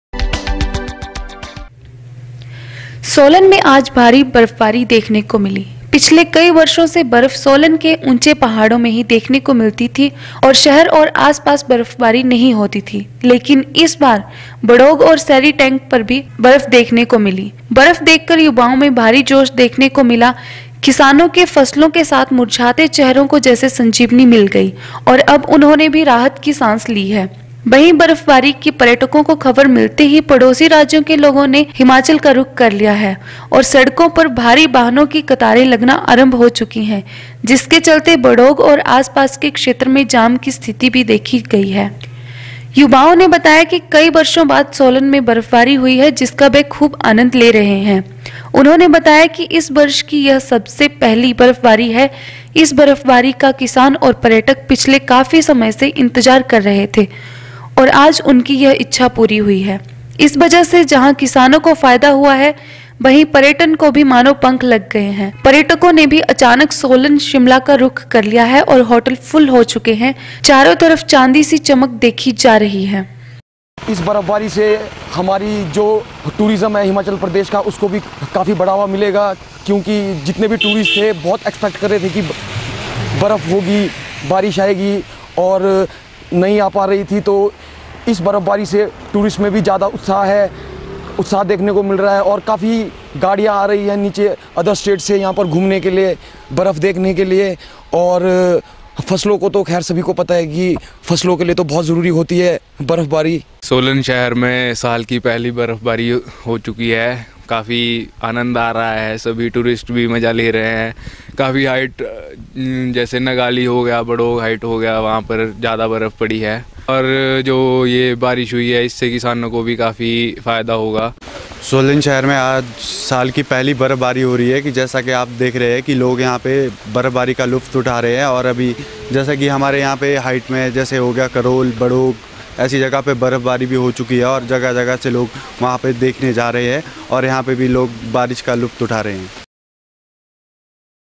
नव वर्ष में किसानों और पर्यटकों की इच्छा हुई पूरी सोलन में हुई जम कर बर्फबारी
नव वर्ष में किसानों और पर्यटकों की इच्छा हुई पूरी
सोलन में हुई जम कर बर्फबारी |
सोलन शहर बड़ोग सेरी ग्लानाग चायल कसौली में भी हुई बर्फबारी |